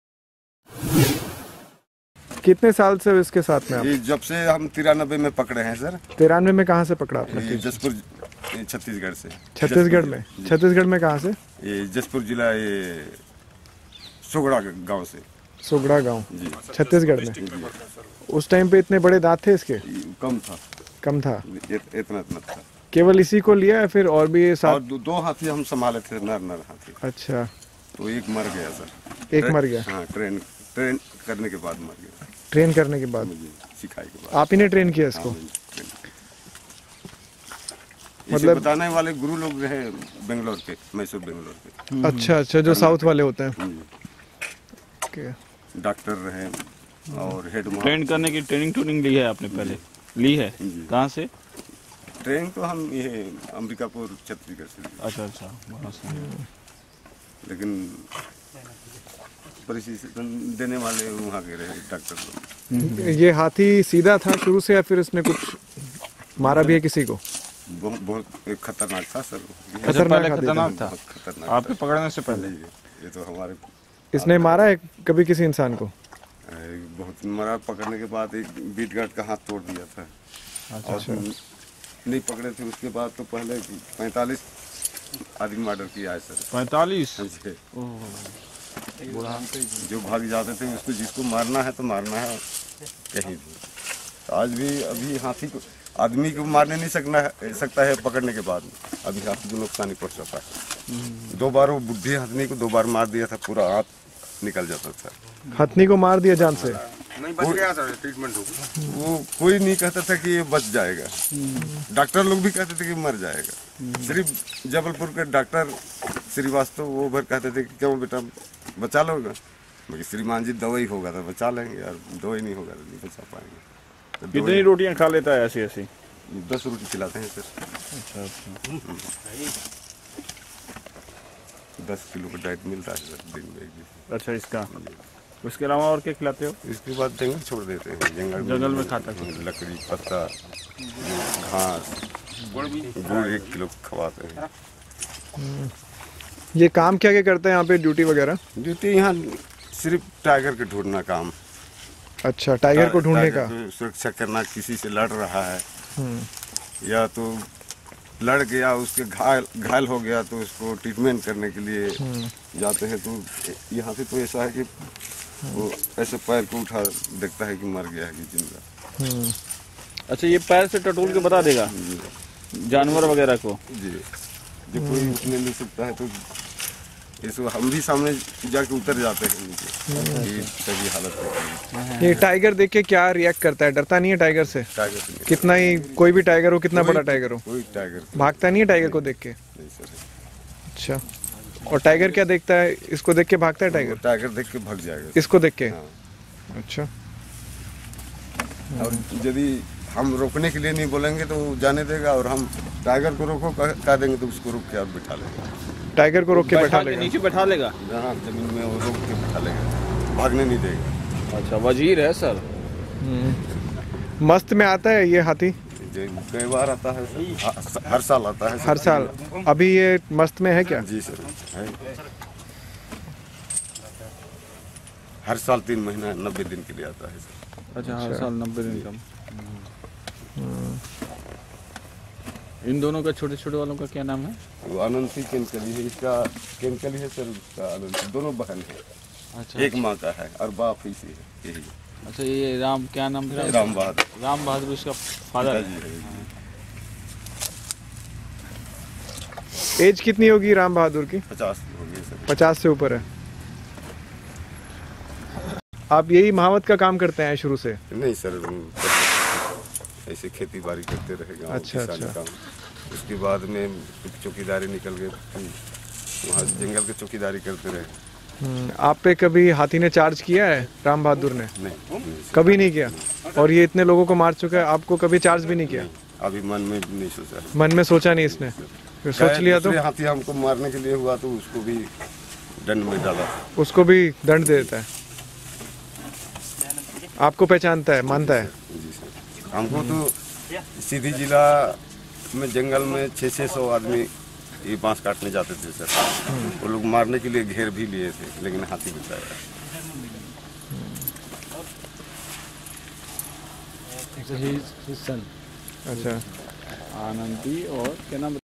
TRAINER OF THE WILD BEASTS | Meet the man who trains the Wild Elephants !
Elephant may be big, but bigger is the will of that tiny man who believes that he can actually control the big beast. These elephant trainers or mahouts spend most of the time understanding the nature of their Elephant and eventually mould the giant to work for them in every situation.

This Elephant works in one of the national parks of India and helps the forest staff to monitor the wildlife and do patrolling efficiently.

This association of man and animal may look unethical, but this job is certainly not for the faint hearted.